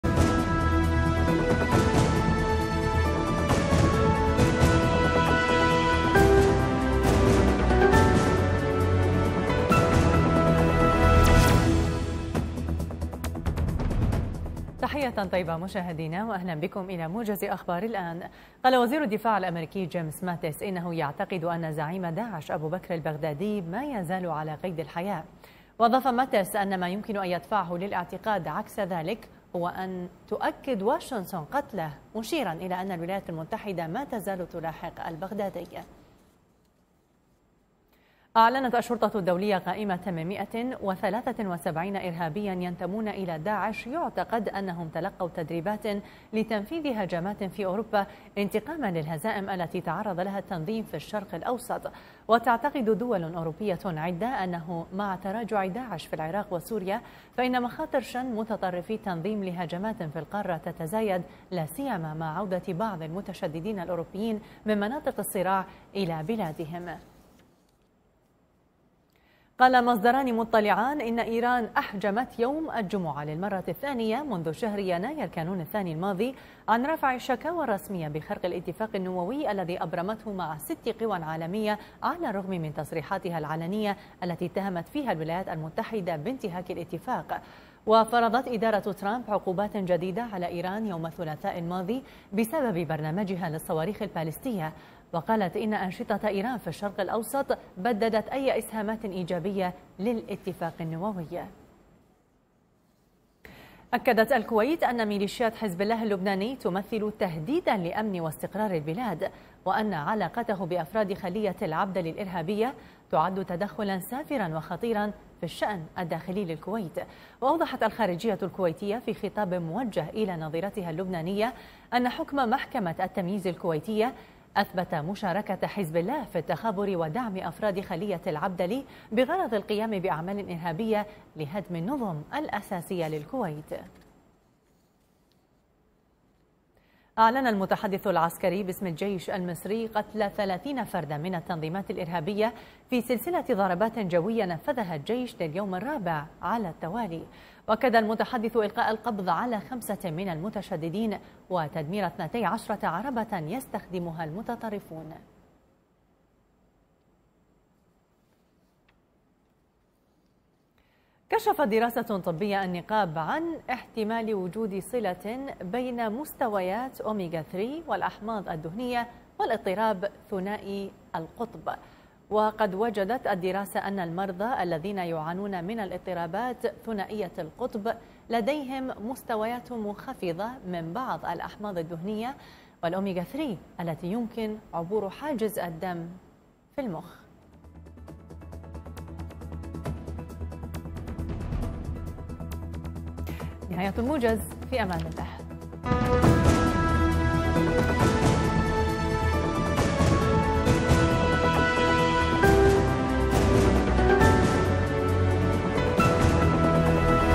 22-07-2017 | موجز الواحدة ظهراً لأهم الأخبار من تلفزيون_الآن‎
العناوين:
- ماتيس يبدي اقتناعه بان أبو بكر البغدادي لا يزال حيا
- 173 داعشيا يهددون
- رويترز: إيران لم تتقدم بشكوى بشأن انتهاك الاتفاق النووى رغم العقوبات
- الكويت تحث الحكومة اللبنانية على ردع
- الجيش المصري يعلن عن عملية عسكرية ضاربة شمالي
- دراسة: الأحماض الدهنية تلعب دورا في اضطراب ثنائي القطب